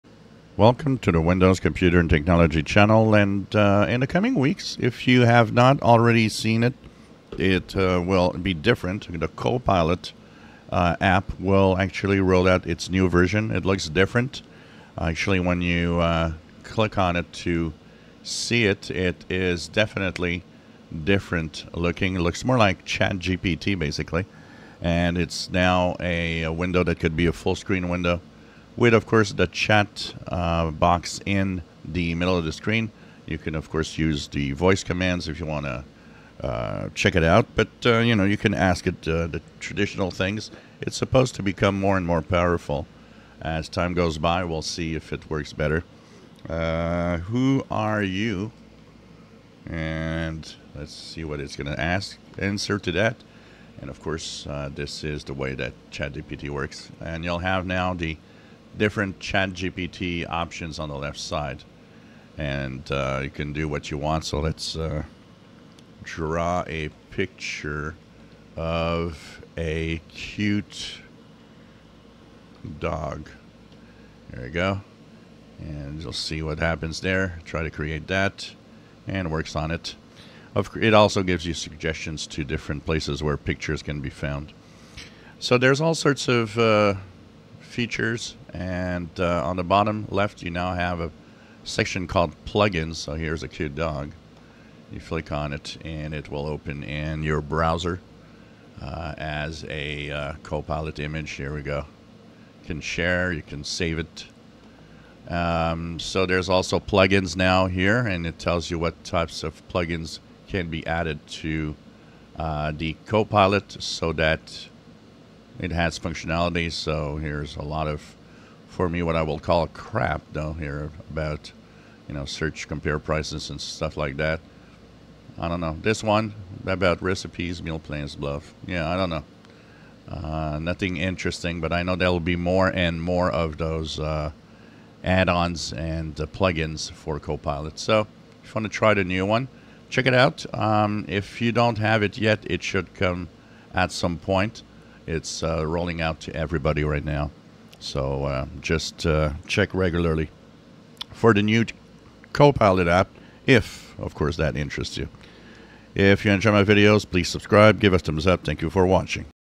Copilot app is getting a makeover and looks more like ChatGPT
New version of copilot is now pushed to more and more Windows PCs